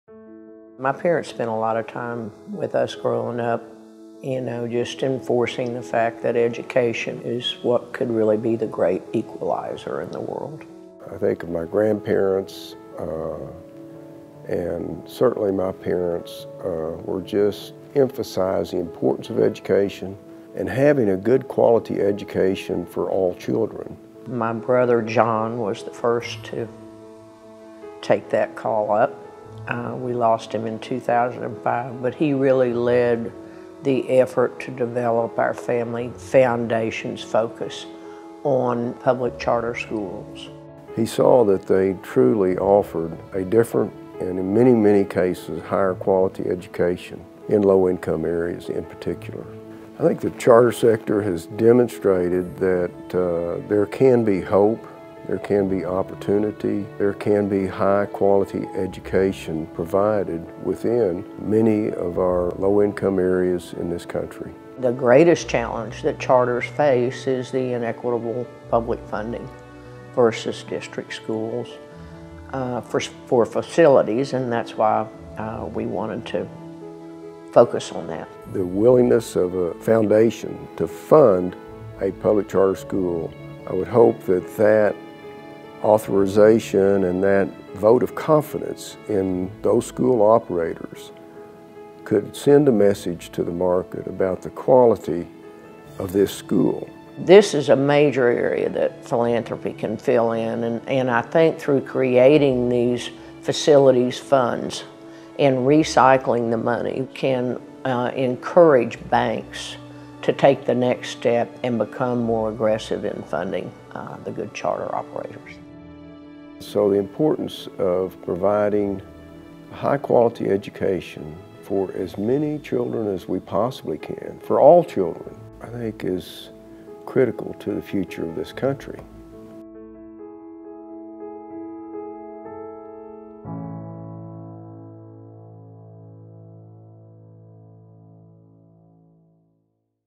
Walton Family Foundation Announces Building Equity Initiative
Alice Walton and Jim Walton explain the Walton family's latest initiative to meet growing demand for high-quality public school options. The Walton Family Foundation announced the $250 million Building Equity Initiative, a first-of-its-kind nonprofit effort to provide charter schools with access to capital to create and expand facilities ›››